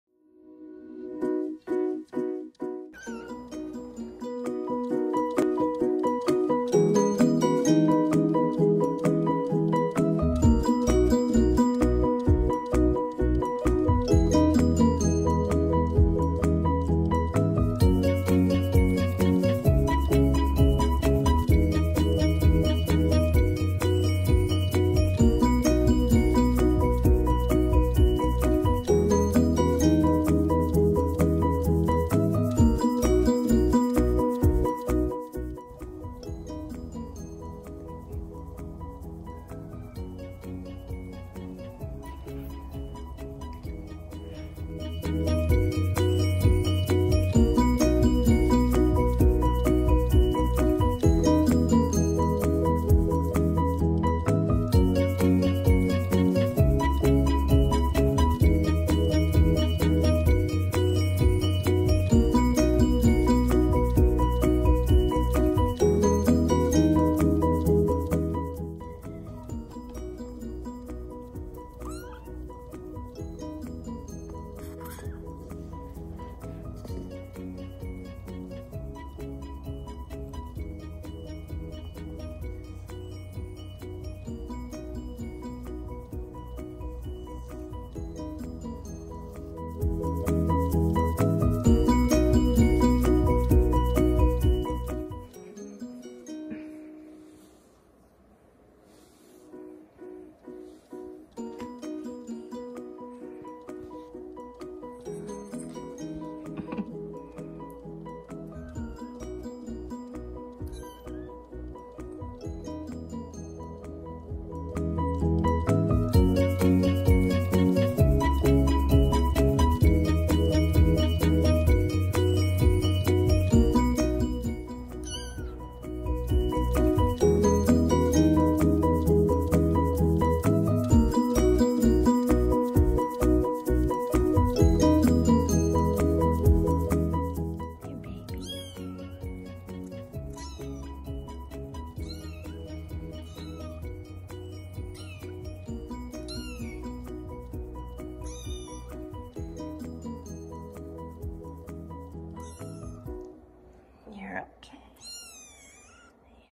Adorable Kitten stays up late for a photo shoot😹😻PRECIOUS Kitten❤️
Sassafras is as FABULOUS as they come! Sweet as sugar and oh soooooo precious. I’m in Love! Reserved, but we have Scottish straights too❤️